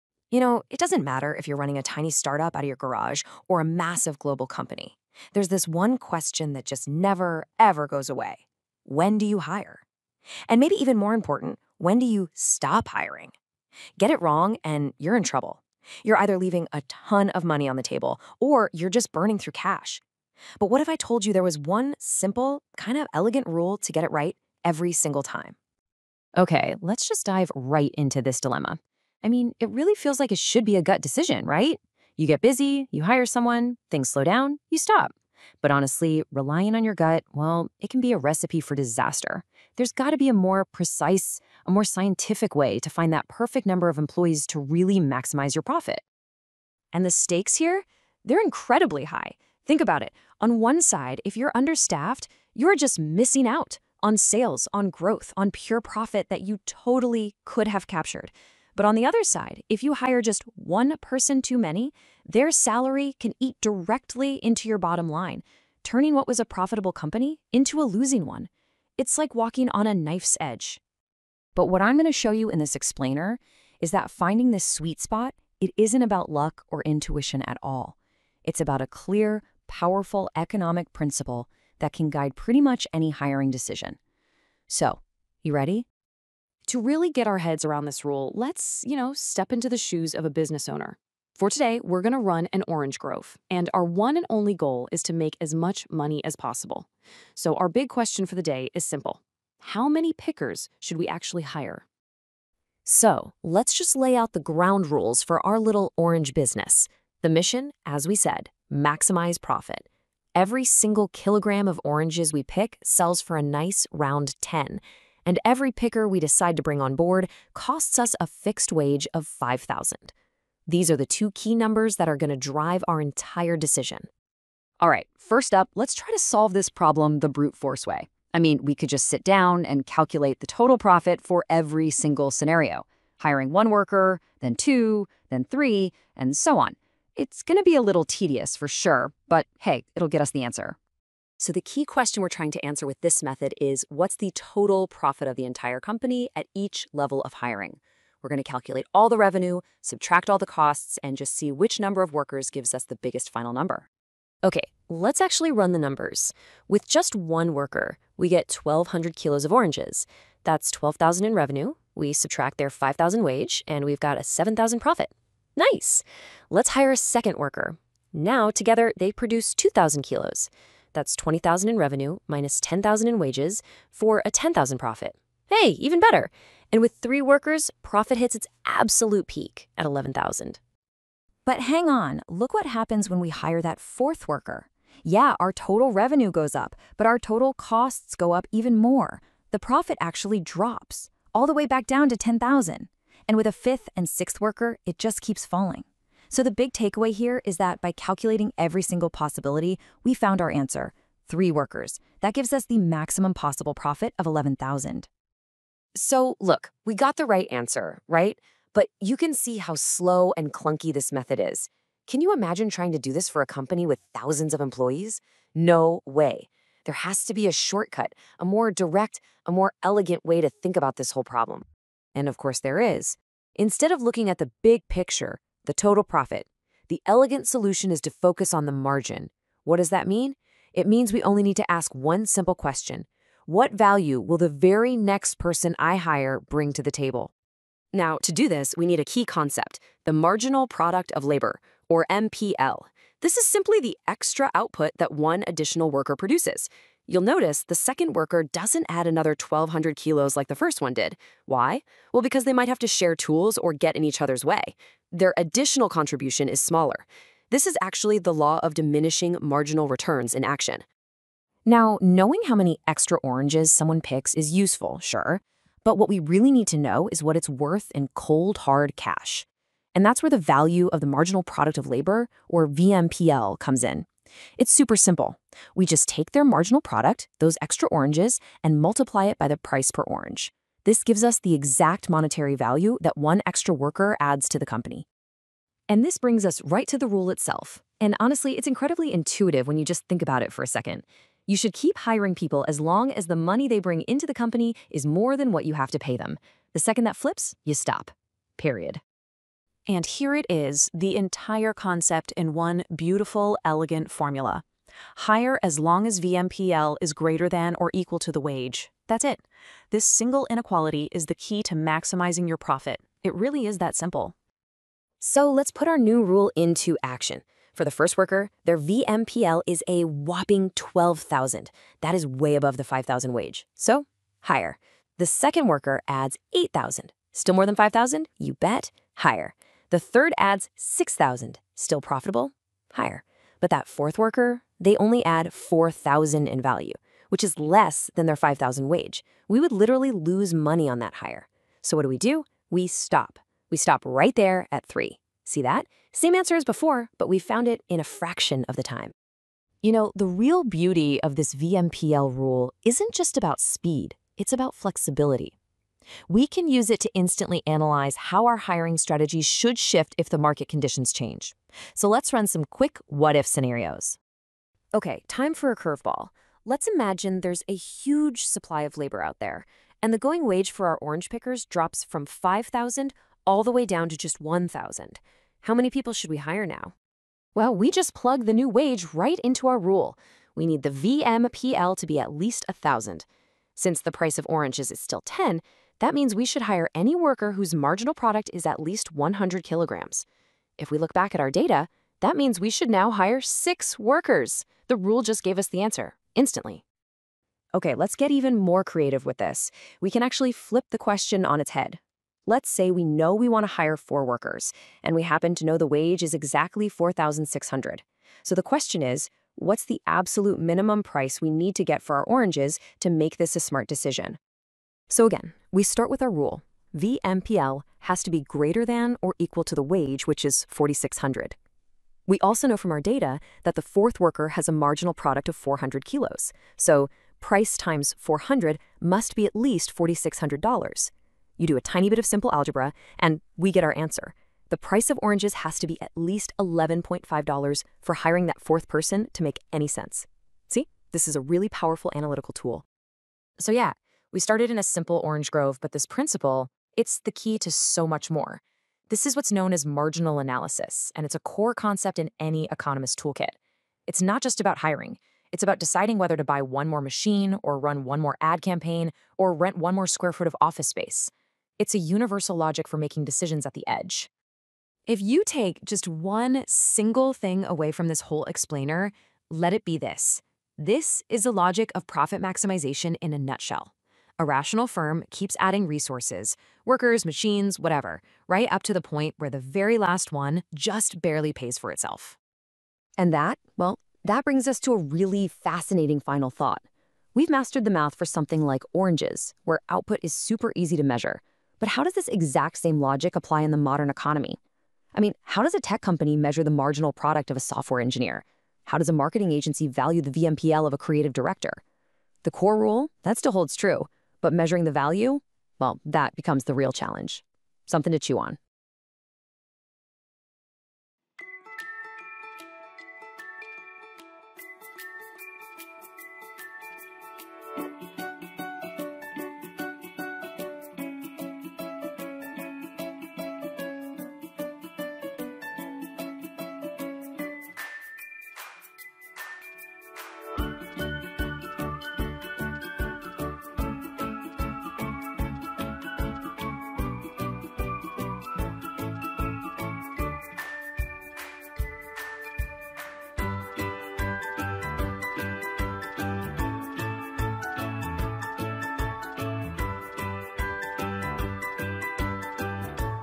Microeconomics: Maximizing Profit for a Producer in Perfect Competition
"This video is part of Michael Raviv’s academic lecture series, providing clear, structured, and reliable university-level education in Economics and Finance to learners worldwide. Invest in yourself."

🎬 Video Description: Profit Maximization | Optimal Labor Using VMPL ≥ Wage

In this microeconomics tutorial, we explore how a perfectly competitive firm maximizes profit by choosing the optimal amount of labor (L). Using a structured production-function dataset, we apply the core hiring rule:

VMPL ≥ W      or P×MPL ≥ W

⚙️ What You’ll Learn:

Optimal Labor Choice: How to determine the profit-maximizing number of workers for given Price (P) and Wage (W).

Wage Decreases: How a drop in wages affects employment decisions.

Required Price: How to calculate the minimum output price needed to justify hiring a specific number of workers.

New Market Data: Working through scenarios with changing prices and wages.

📚 Practice Included:
At the end of the video, you’ll find clear tables summarizing all calculations. Pause and practice to master the VMPL hiring rule!